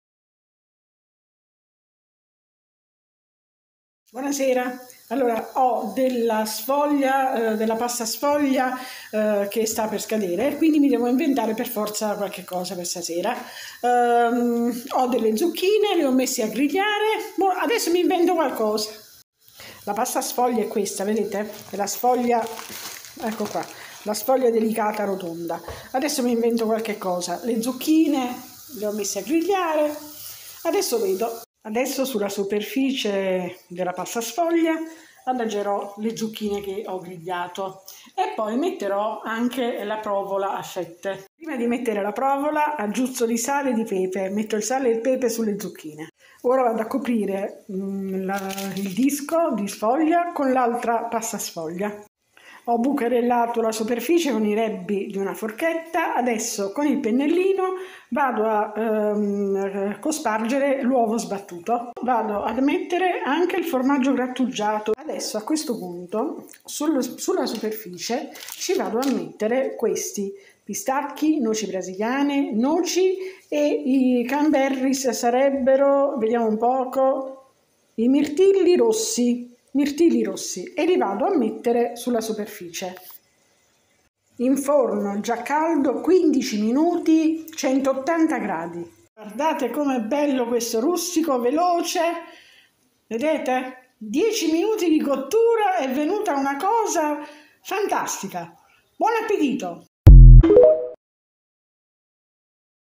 sfogliata con le zucchine.
buongiorno a tutti. oggi sfogliata con le zucchine
 pasta sfoglia, zucchine grigliate, formaggio grattugiato sale pepe e formaggio a fettine a piacere, uovo, semi vari a piacimento. e se vi fa piacere iscrivetevi al mio canale 👍 perché chiù ne simm e chiù bell parimm